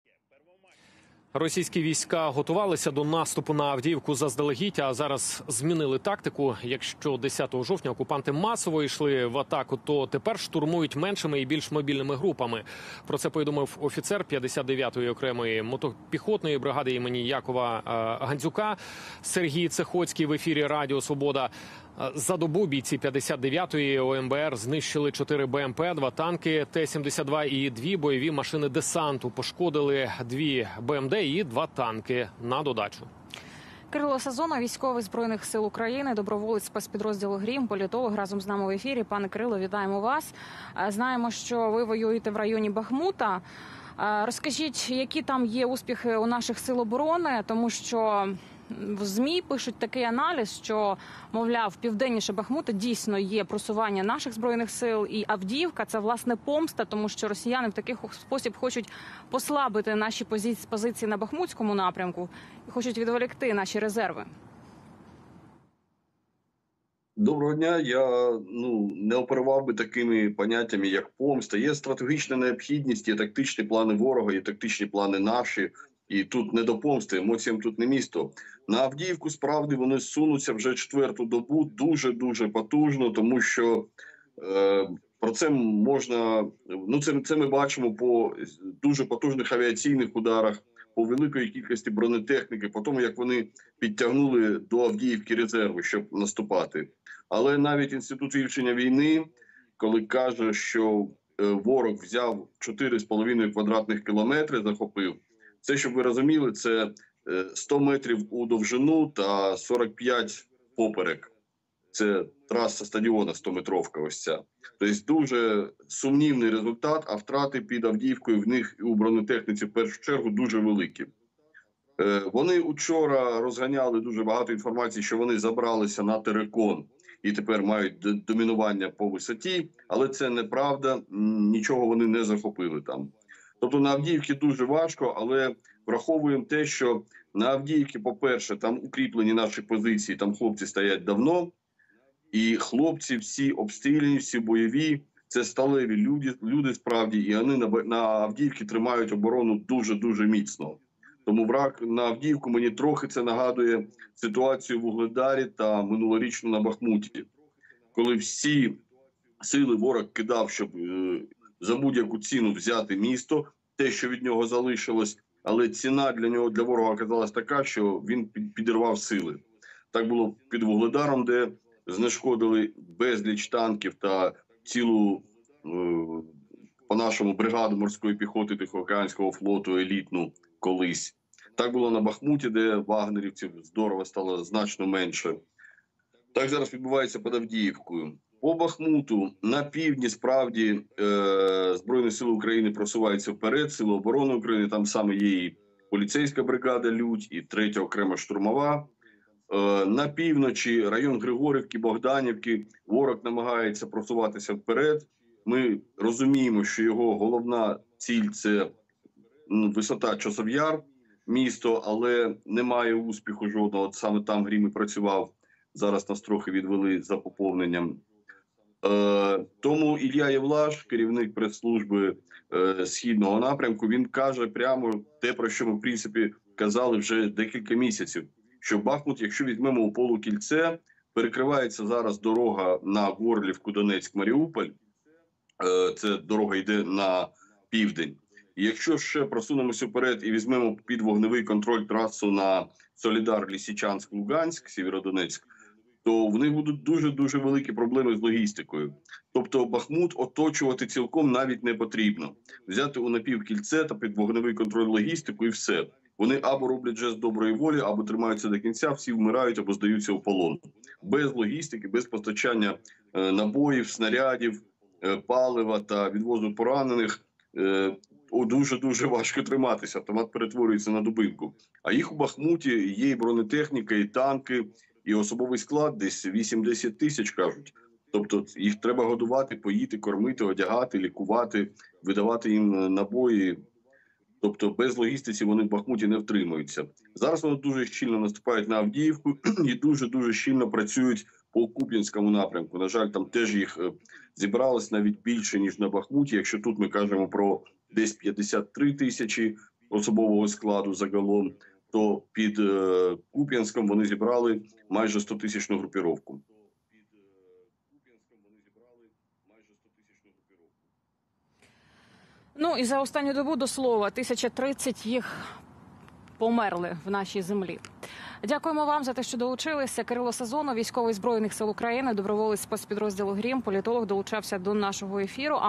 СТЯГУЮТЬ ВСЕ НА АВДІЇВКУ! САЗОНОВ: ситуацію під Авдіївкою схожа на ту, що була з БАХМУТОМ
Військовослужбовець ЗСУ Кирило Сазонов про ситуацію поблизу Авдіївки.